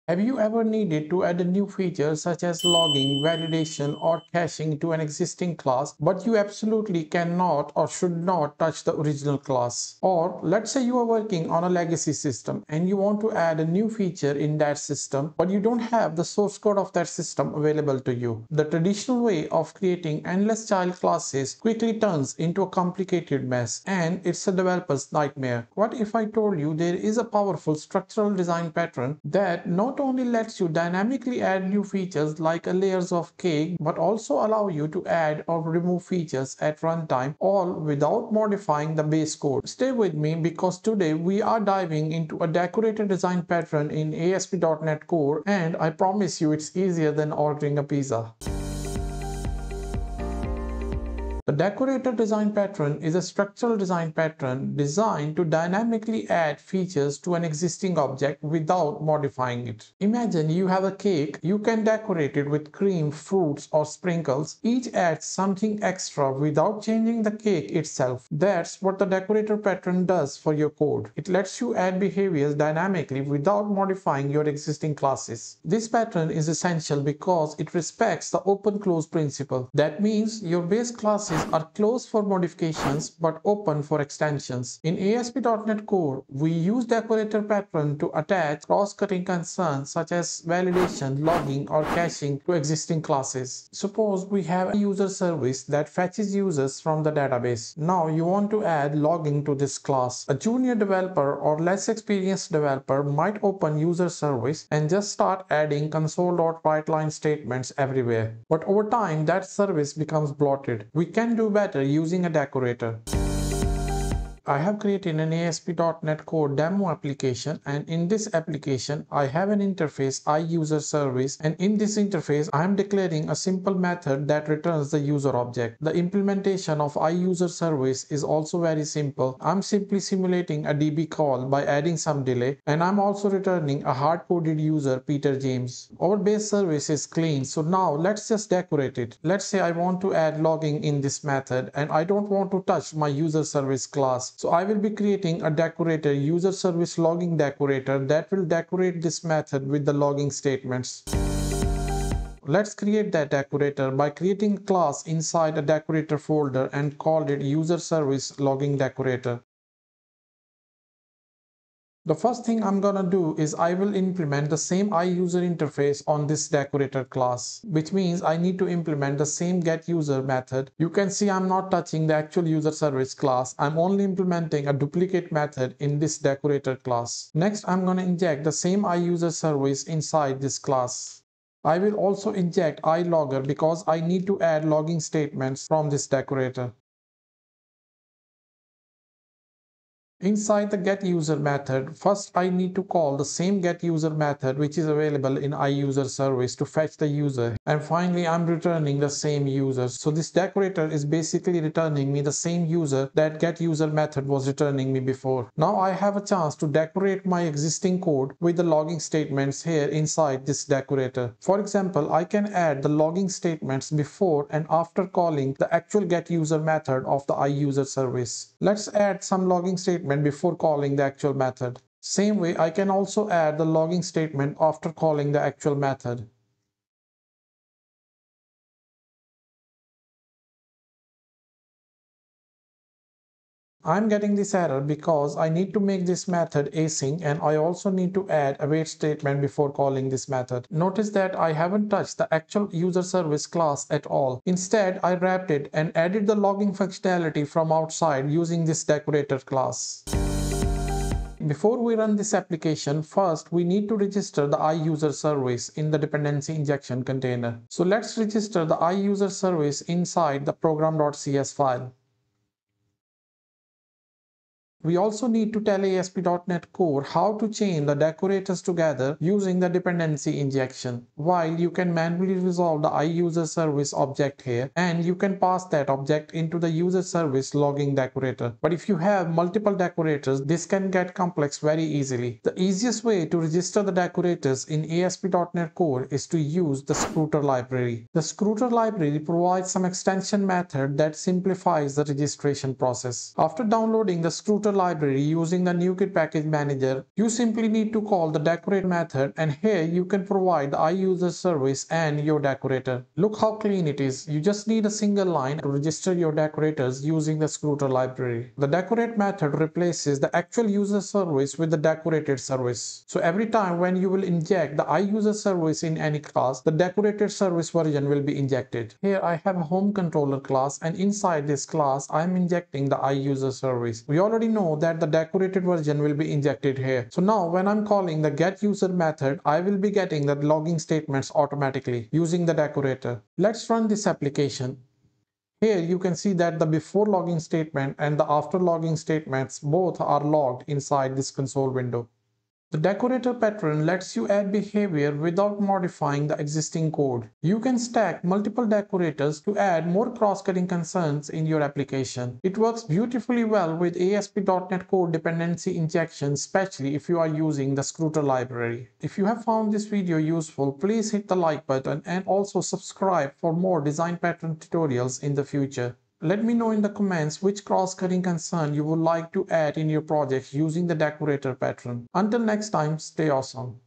Decorator Pattern in ASP.NET Core: Add Features Without Changing Code
Learn how to use the Decorator Design Pattern in ASP.NET Core to add cross cutting concerns such as logging, caching, or validation — without touching your existing code!

🎥 In this video, you’ll see how decorators help keep your application clean, modular, and scalable.

💡 What You’ll Learn
✔️ What is Decorator Pattern
✔️ A real-world coding example
✔️ Adding Logging using Decorator
✔️ Registering decorators in ASP.NET Core

📰 My Blog Posts on Decorator Pattern
➡️  Decorator Design Pattern in ASP.NET Core

⏱ Chapters
⏱ 00:00: Introduction
⏱ 00:51: What is Decorator Pattern?
⏱ 01:52: A Coding Demo
⏱ 02:31: Implementing a Decorator class
⏱ 04:10: Registering decorators in ASP.NET Core
⏱ 05:42: Recap